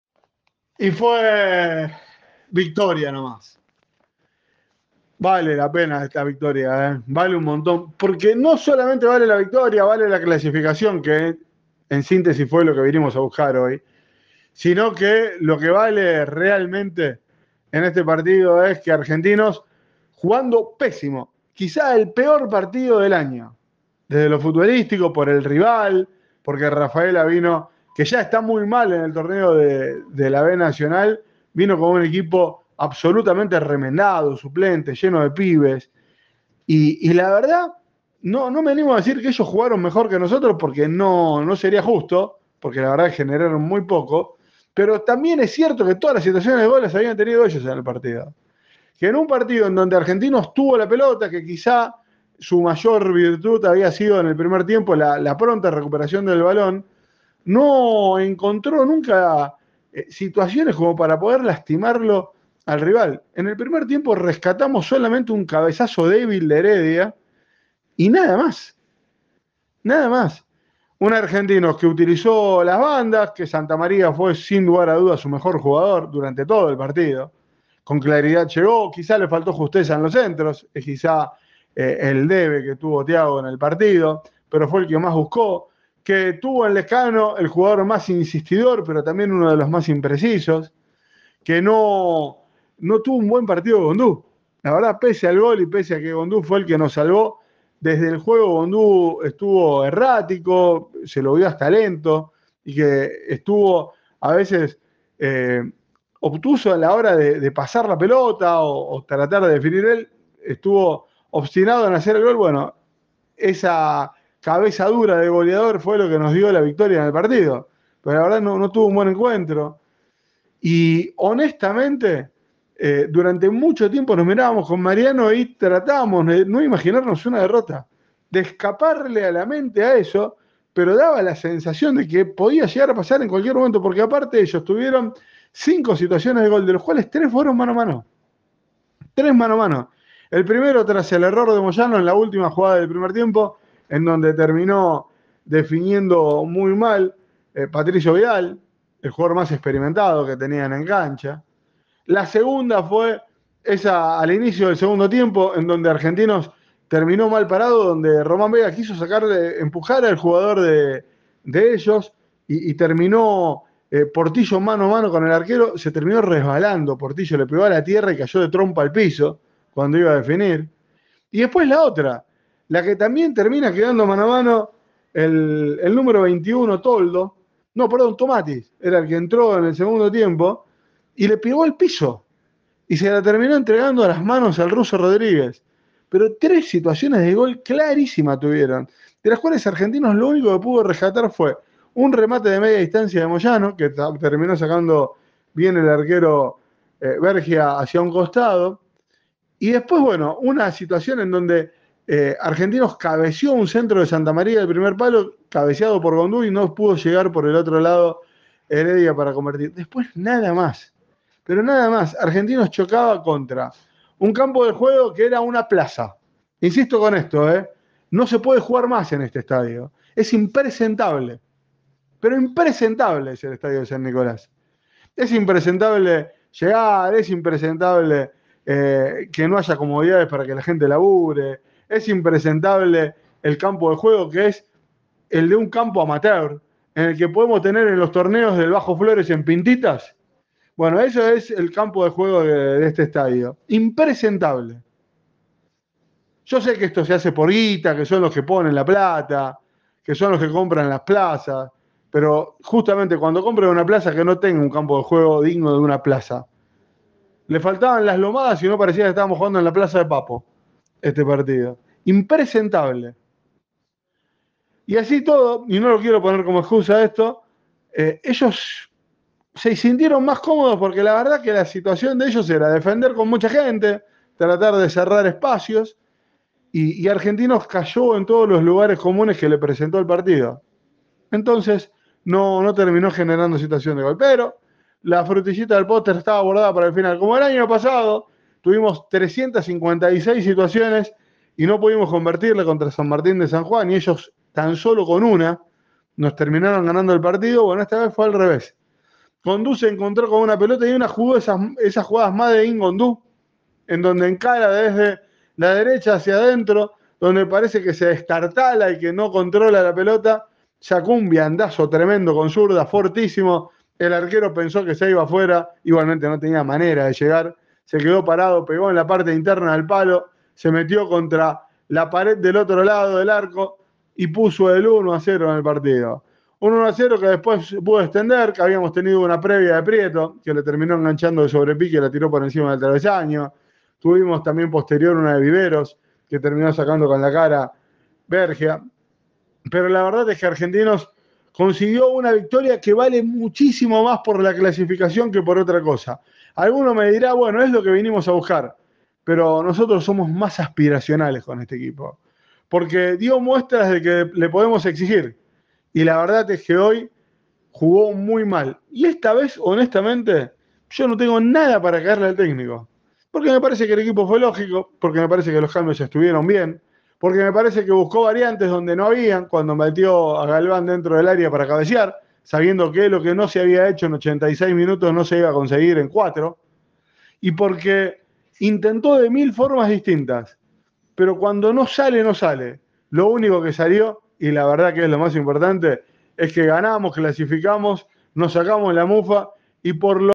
POCO PARA RESCATAR | El comentario de David Bursztyn tras la victoria de Argentinos ante Rafaela
Defensa
Defensa y Justicia
Copa de la Liga
Argentinos
AAAJ
Argentinos Juniors